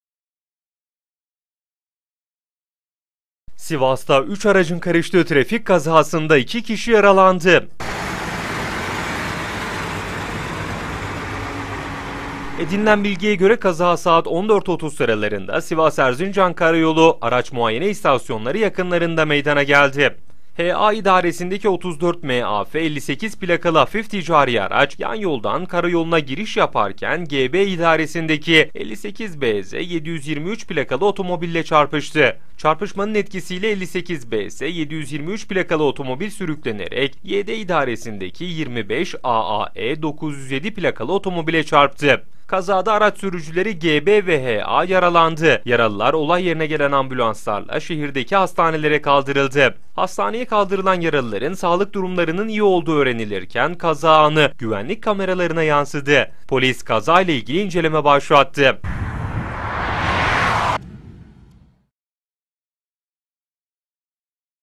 SİVAS'TA ÜÇ ARAÇ ÇARPIŞTI: 2 YARALI
SİVAS'TA ÜÇ ARACIN KARIŞTIĞI TRAFİK KAZASINDA 2 KİŞİ YARALANDI.

Edinilen bilgiye göre kaza, saat 14.30 sıralarında Sivas-Erzincan karayolu araç muayene istasyonu yakınlarında meydana geldi. H.A. idaresindeki 34 MAF 58 plakalı hafif ticari araç, yan yoldan karayoluna giriş yaparken G.B. idaresindeki 58 BZ 723 plakalı otomobille çarpıştı.